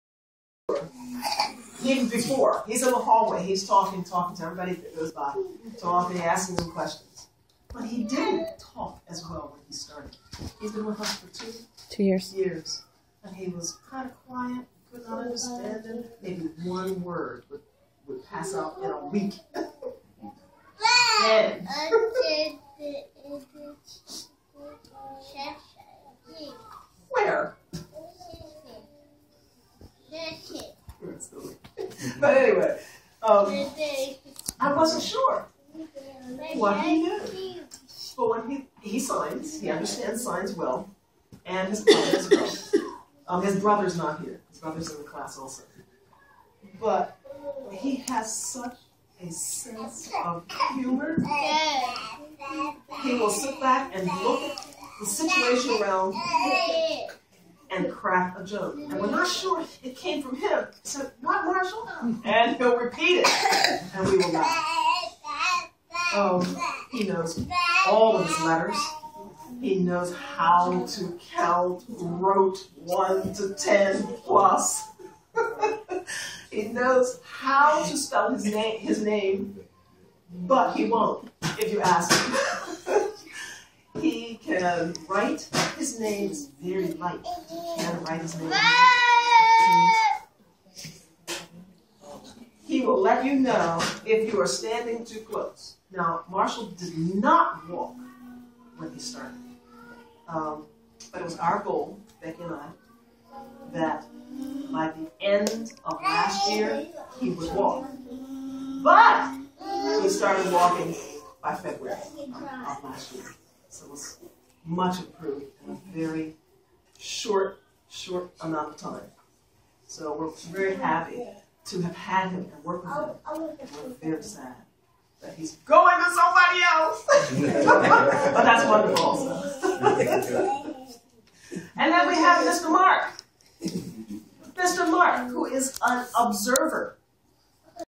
Marshall's Pre-K Graduation
Mrs. Henry says a few words at the Villa Cresta pre-K graduation day ... she's awesome :)